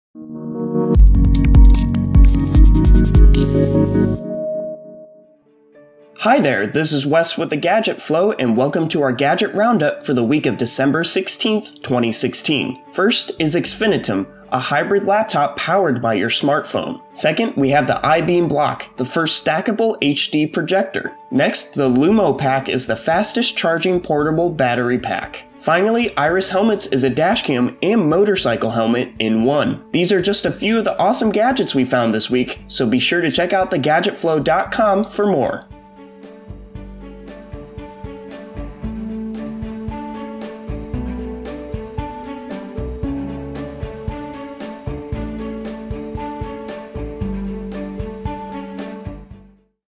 Weekly Gadget Flow Roundup - December 15th 2016
XFINITUM – Hybrid Laptop Powered by Your Smartphone

Give your smartphone the additional screen space, storage & power it requires with XFINITUM. This is a hybrid laptop powered by your smartphone. With one single touch, XFINITUM enables you to transform your smartphone into a full laptop replacement. You can connect it wirelessly to any iOS or Android Smartphone, and the XFINITUM will use your Smartphone as a processing unit. Unlike regular laptops and smartphones, XFINITUM is capable of working with multiple devices and different operation systems. So you will be able to watch movies, play games, browse the internet, and play music on the laptop while the content gets processed by the computing power of your smartphone. All you have to do is download the XFINITUM app on your phone, activate the “pair” feature in the app to set up the wireless connectivity and you’re ready to go. The biggest advantage of the Xfinitum device is that you won’t have to upgrade it anytime soon. Since it’s being run by your smartphone, the only device you’ll have to upgrade is your smartphone. From playing mobile games to multiple consoles, XFINITUM will take your gaming experience to new heights altogether. With a device storage of up to 1TB and a battery life of 73 hours on a single charge, this is a laptop you’ll surely not wish to part with!

LumoPack – The Fastest Charging Portable Battery Pack

Get back to 100% on the go with LumoPack, the fastest charging portable battery pack. This incredible 6000 mAh power pack takes just six minutes of charging at 140 Watts to have enough juice to give your iPhone 6S a full charge. In less than 18 minutes, the LumoPack can be fully charged to 100% and ready to take on the road. The coated lithium ion materials of the LumoPack can last though over 1000 cycles so you never have to worry about a low battery on your device again. Included with the LumoPack along with three methods of charging. The LumoPack is the fastest charging battery pack and will give you peace of mind when you’re out and away from an outlet such as in your car or camping.

IRIS Helmets – Dash Cam Motorcycle Helmet

The IRIS helmet is designed for the rider seeking safety and protection in the case of an accident. Recording in real-time for every part of your journey, the DC-2 always has your back. Vision and protection are effortlessly integrated into a new level of safety. With dual 1080p cameras recording in 30fps, the IRIS DC-2 is the best way to capture your ride. The cameras each provide you with a 135-degree field of view to capture everything going on behind and in front of you. The high definition footage allows for the identification of number plates and the confidence of knowing that everything going on around you is being recorded. The camera hardware has been engineered not to compromise the structural integrity of the helmet. In the event of an accident, the cameras are pushed up and away from the cranium, unlike externally-attached devices. These add-on devices compromise the impact absorbing ability of the helmet by putting all the force into a concentrated area. Larger, side-mounted cameras can also act as catch points when sliding along the road, increasing the risk of the rider’s head being twisted and pulled into a dangerous position. Now that’s a perfect accessory for effortless, safety-focused ride capturing.

iBeamBLOCK Projector and Multimedia Device

Inspired by LEGOS, the iBeamBLOCK is the first stackable HD projector with a Win10 tablet and power bank combined. It makes displaying all your digital content and multimedia a cinch! iBeamBLOCK’s modular design comes packed with everything (Wi-Fi, Bluetooth, USB, MicroUSB, MicroSD card, HDMI interface, etc.) you need to seamlessly go from a killer office presentation to a personal entertainment powerhouse. Plus it all connects (magnetically) without any cords! The idea was to make DLP projectors fun and creative which is why this design has been inspired by the LEGO toy bricks. The projector can display Excel, Word, PowerPoint and PDF documents with 400 lumens of brightness. The Manhattan-type DMD array technology ensures that you get access to crystal clear projection quality of texts & lines. Currently crowdfunding on Kickstarter, you can get your iBeamBLOCK on Kickstarter starting at a pre-order price of $599. Delivering presentations anywhere will now be super easy.